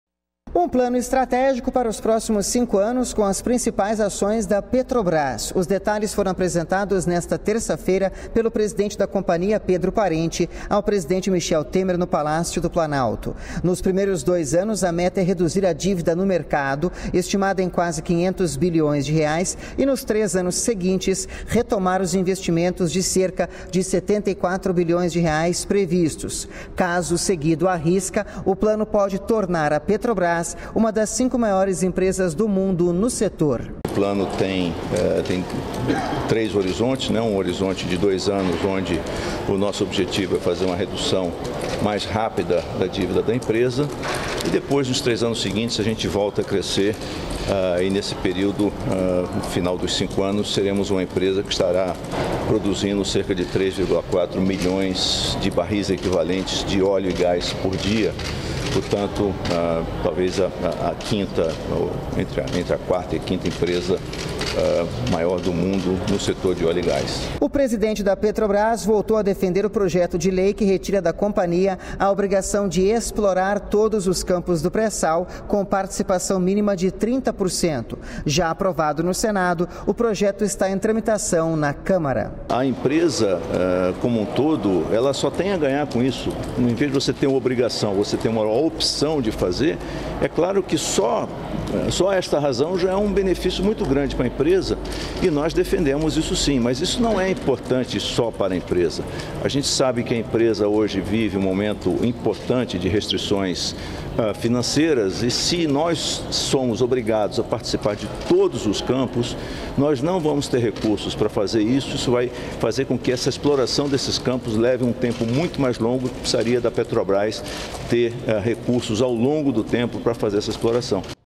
Petrobras petende reduzir dívidas e aumentar produção nos próximos anos
REPÓRTER NBR – 27.09.16: O presidente da Petrobras, Pedro Parente, apresentou o Plano estratégico da estatal ao presidente Michel Temer. Objetivo do plano é reduzir déficit da estatal, permitindo que ela volte a crescer até 2021.